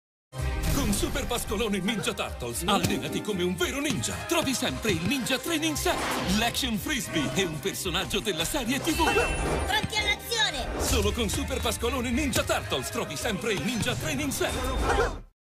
Giochi Preziosi - Tartarughe Ninja - Pasqualone - Uovo - IT
Il Super Pasqualone Ninja Turtles è uno speciale contenitore a forma di uovo, ideato da Giochi Preziosi. È perfetto per rendere unico il giorno di Pasqua a tutti i bambini amanti delle Tartarughe Ninja. Gioca e scopri tutte le sorprese contenute all'interno dello speciale uovo. Troverai sempre la ninja training bag (pungiball gonfiabile da terra) con cui il bambino potrà allenarsi, la sacca zaino personalizzata, completamente realizzata in tessuto impermeabile (completa di lacci da spalla), il frisbee e un originale personaggio giocattolo a sorpresa -alto 10 cm- con braccia e gambe snodate.